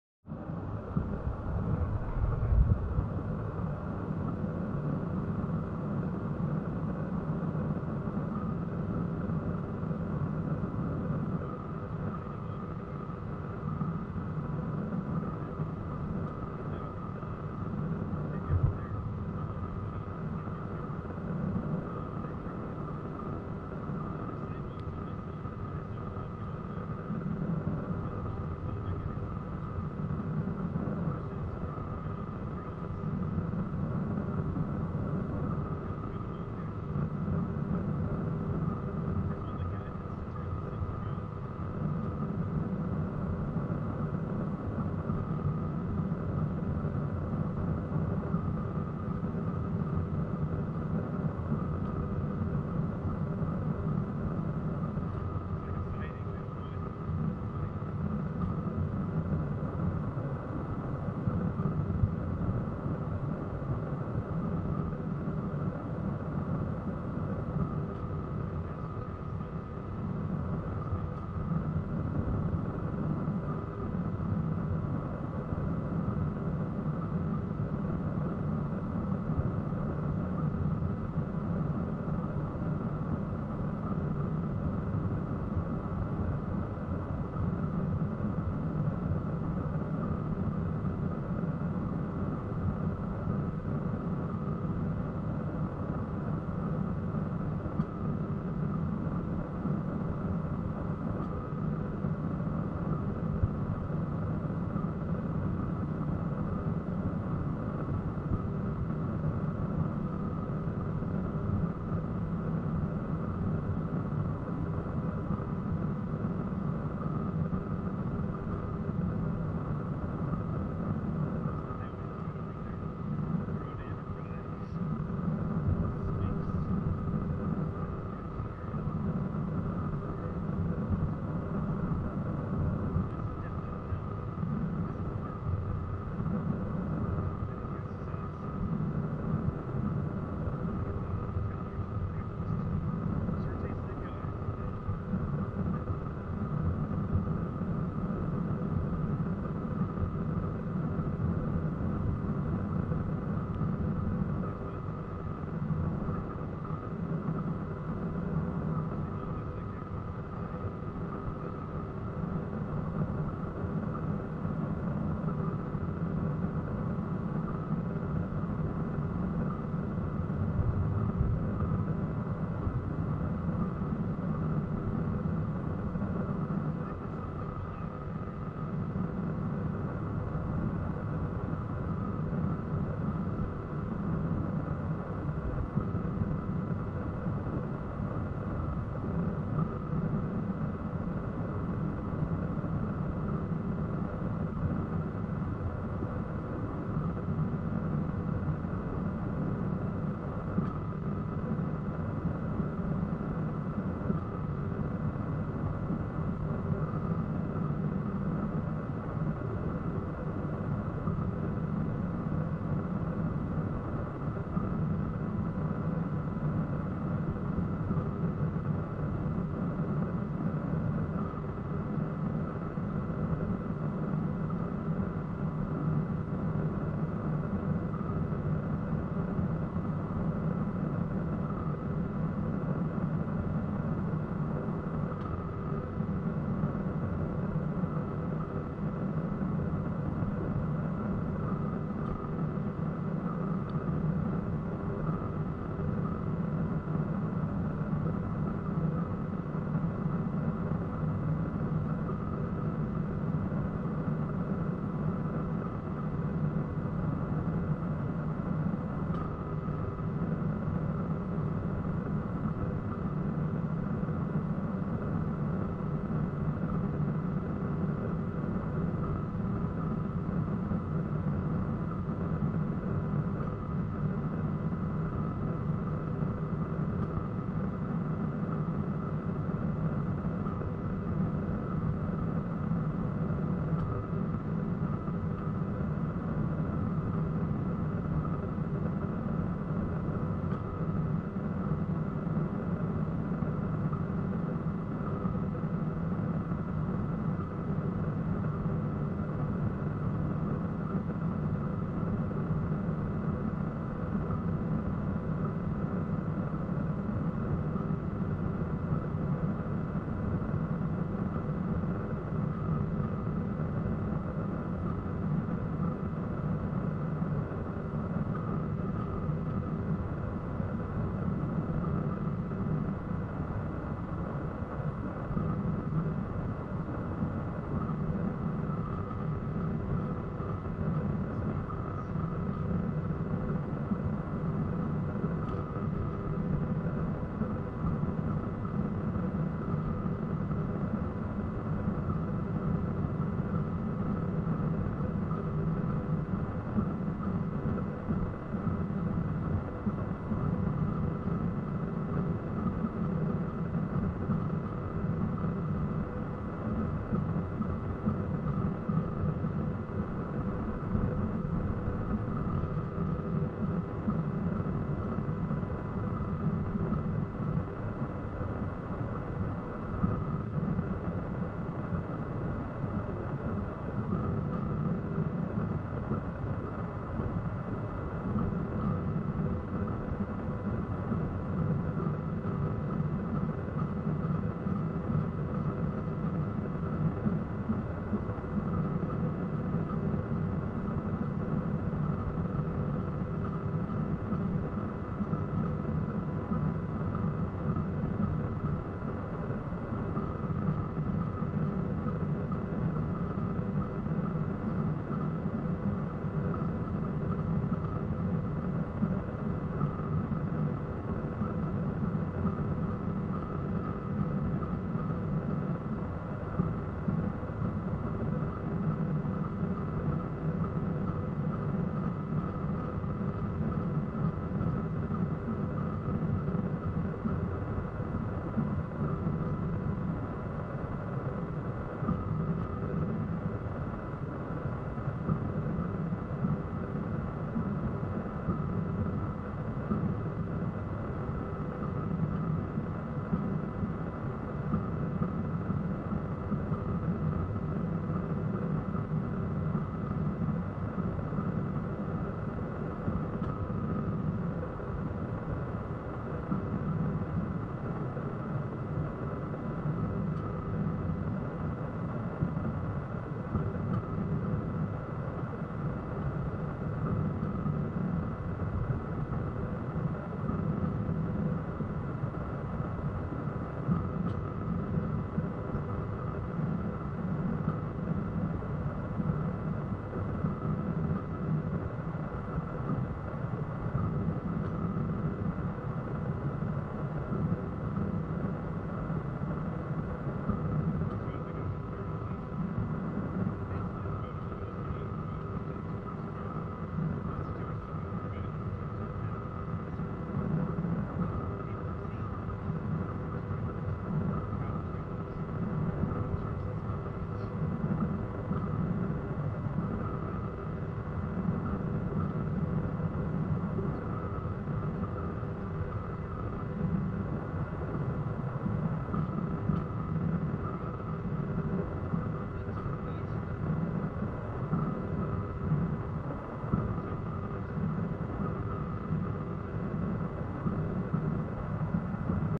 Canyon Twist, video 1, part 1, 2160p60
This is the "Canyon Twist" trip on the Joseph Branch Railriders.  This trip starts at Minam, Oregon and goes east along the Wallowa River then returns the same way.